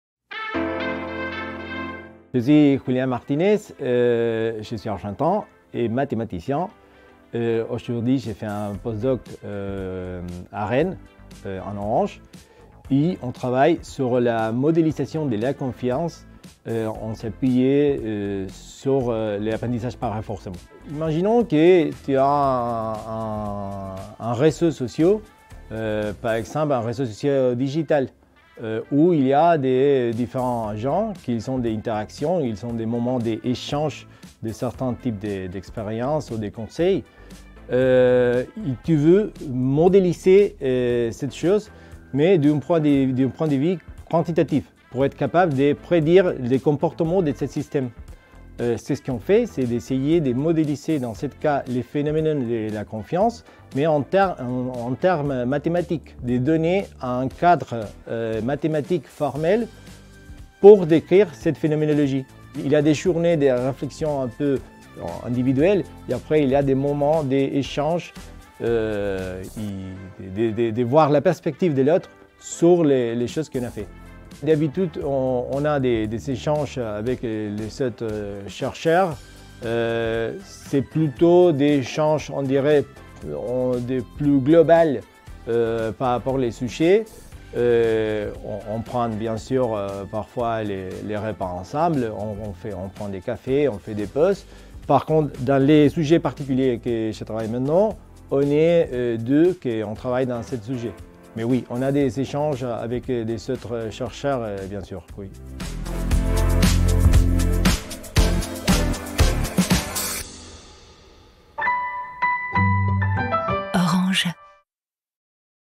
Julian, postdoc chez Orange
Julian nous présente son métier de mathématicien et son parcours. En tant que post-doctorant, il nous parle de modélisation de la confiance et de son quotidien chez Orange.